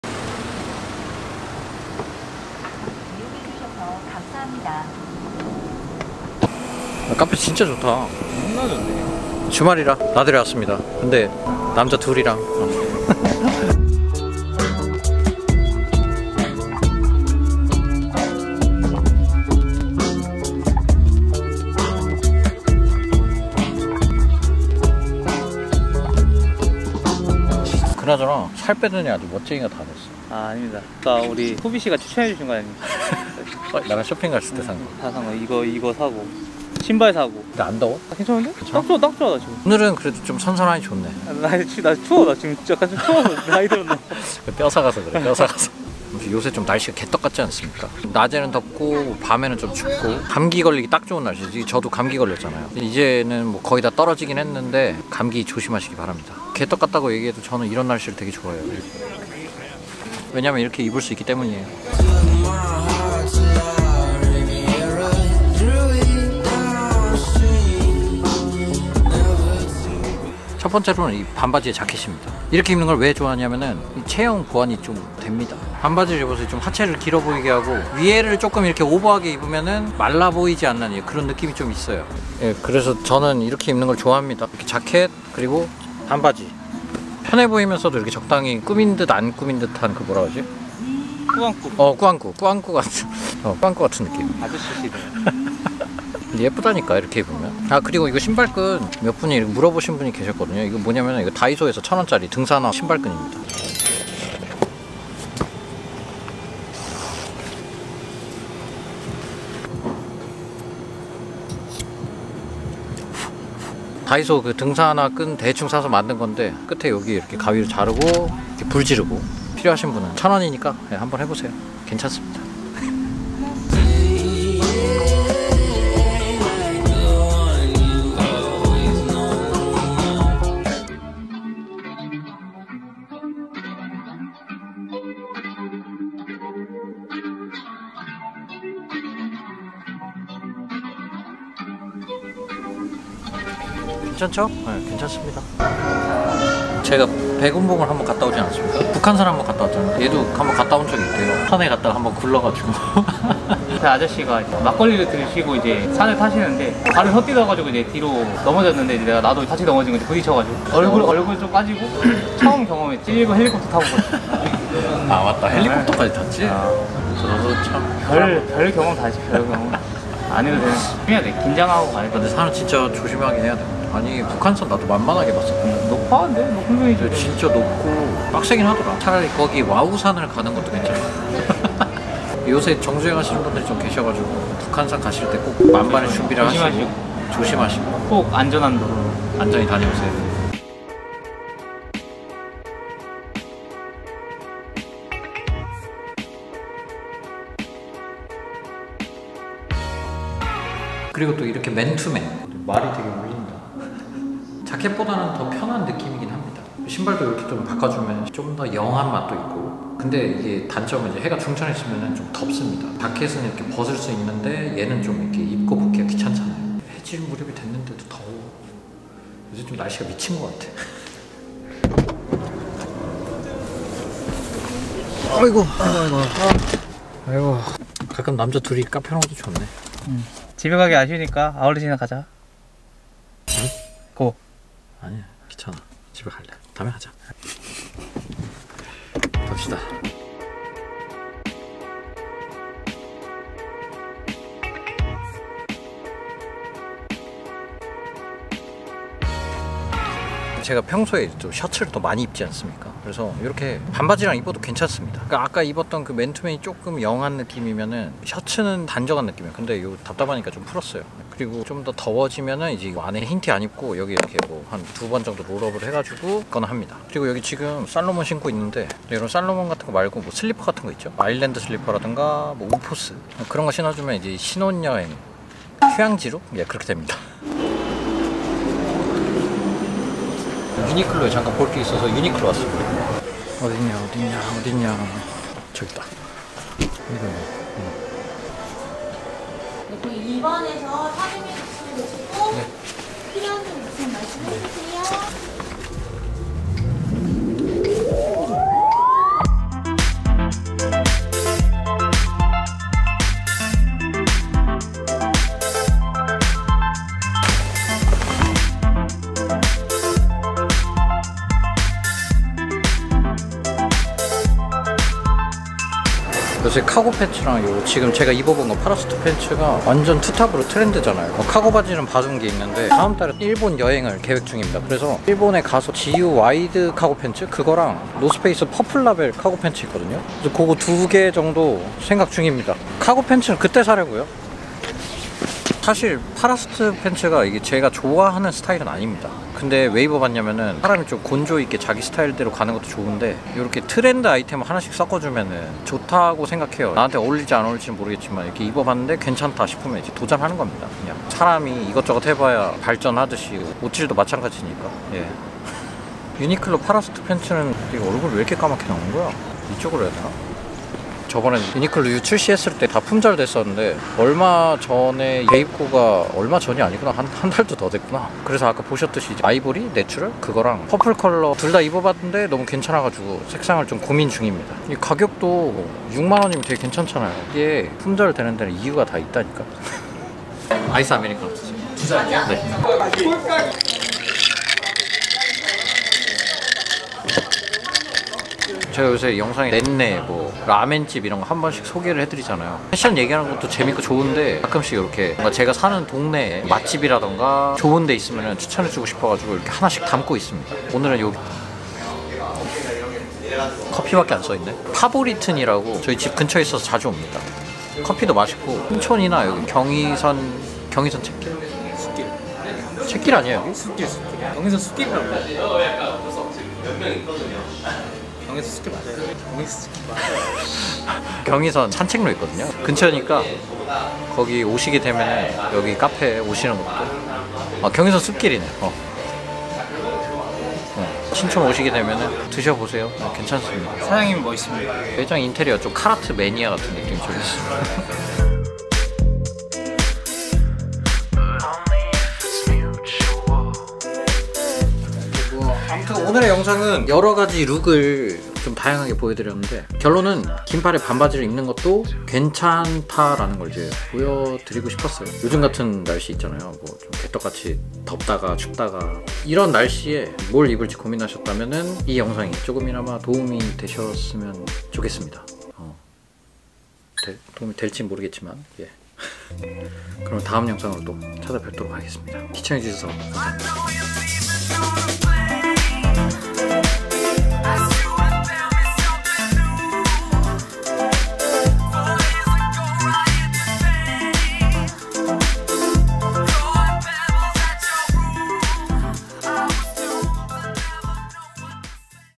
요새 입으면 딱 좋은 코디
00:00 [인트로]

01:02 [자켓 + 반바지 01]

02:05 [자켓 + 반바지 02]

03:10 [스웨트셔츠 + 반바지]

04:00 [셔츠 + 반바지 ]

04:57 [유니클로 파라슈트 팬츠]

06:21 [카페 파보리튼]

07:24 [결론]

Gear.

music.